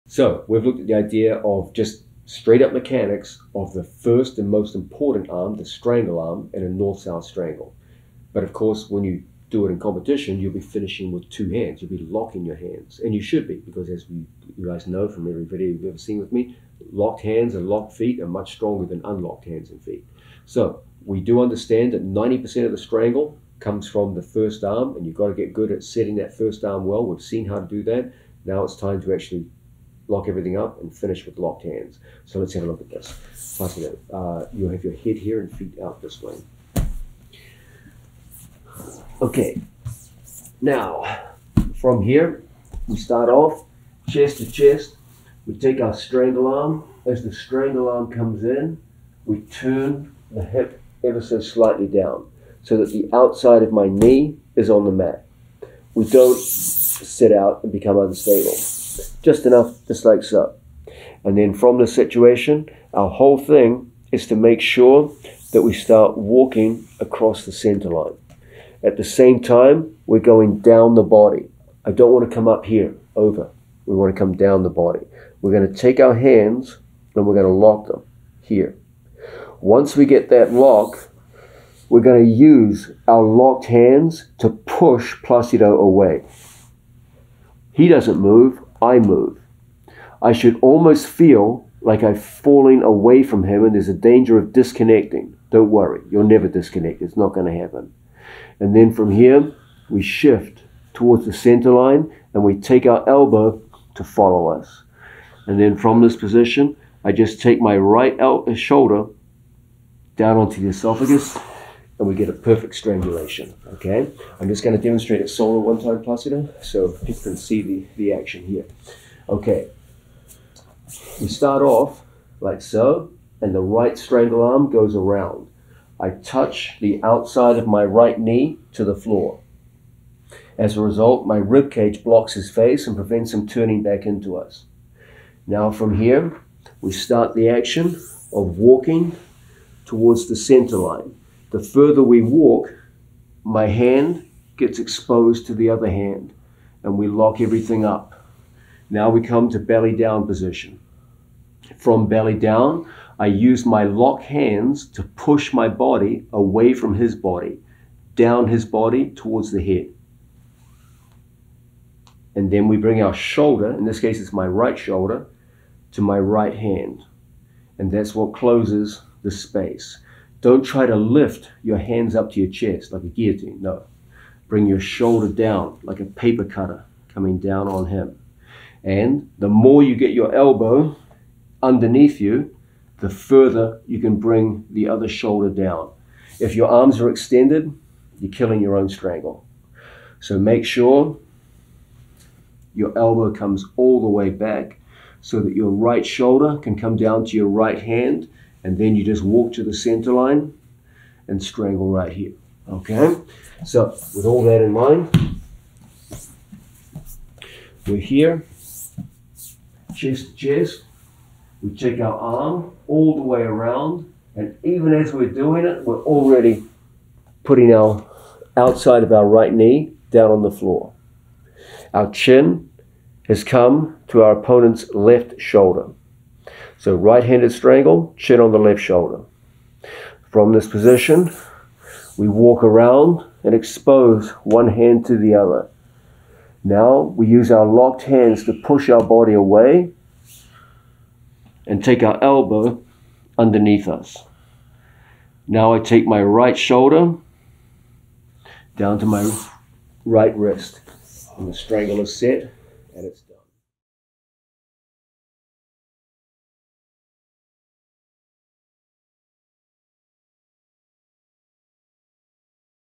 North/South Strangle - Finishing With 2 Hands by John Danaher
FINISHING the North/South Strangle with 2 HANDS

John Danaher demonstrates how to finish a North/South Strangle with 2 Hands in this video.

John Danaher is widely regarded as the greatest Jiu Jitsu Coach of all time.  This is an excerpt from AGELESS JIU JITSU TOP GAME NO GI available from BJJFanatics.com.